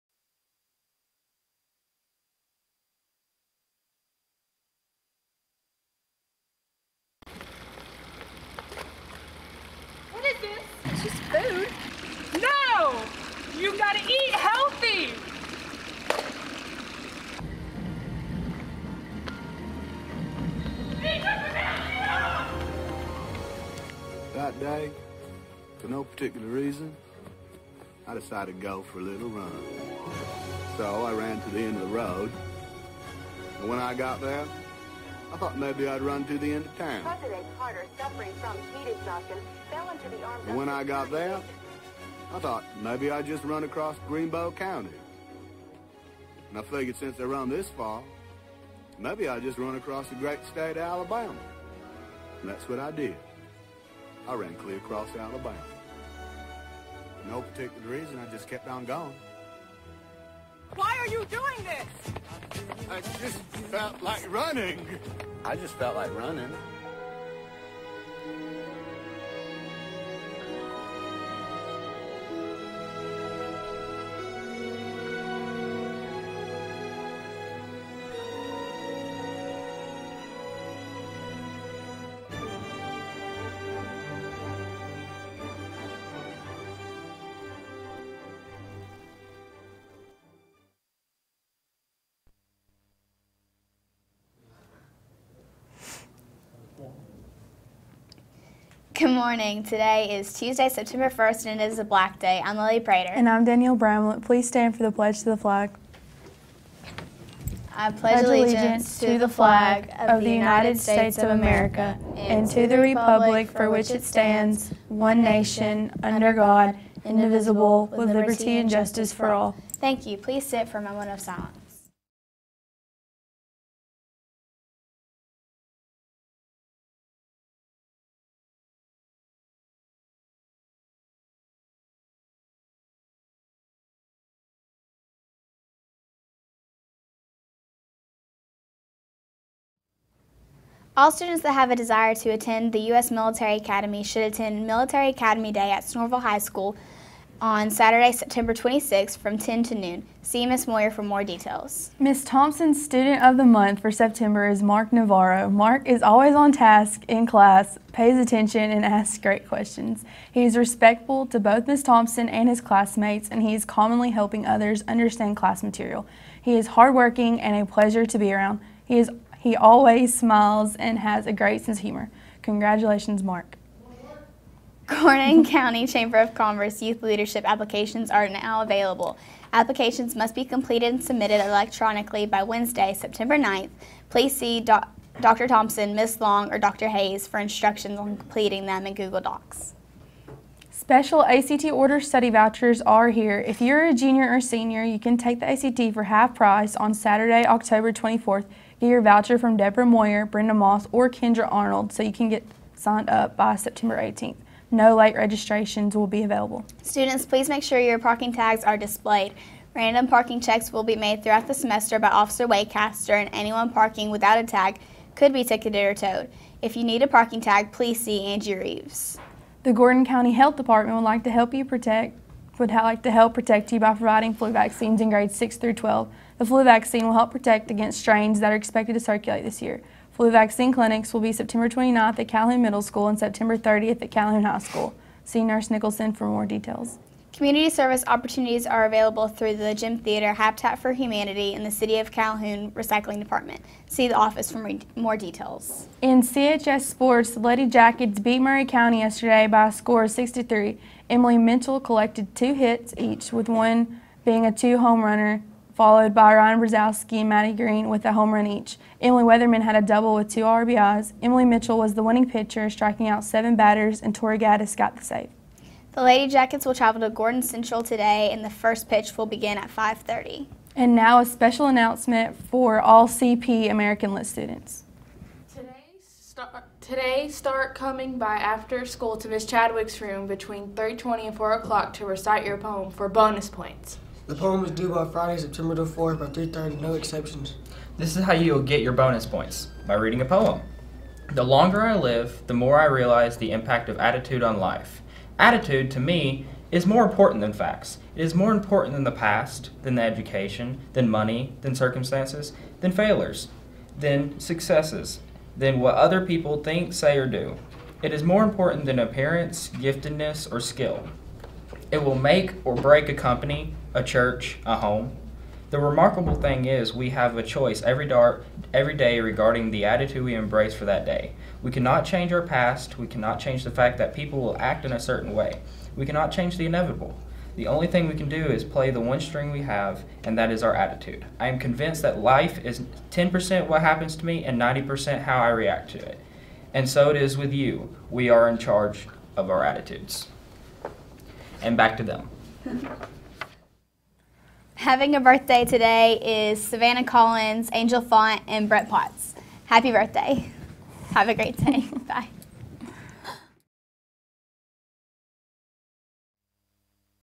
September 1st CHS Morning Announcements
Calhoun High School Announcements for September 1st 2015 featuring SUPER BANANA MAN and lots of CHS news and sports!!! Want BONUS points in American Lit? See the announcement about reading a poem!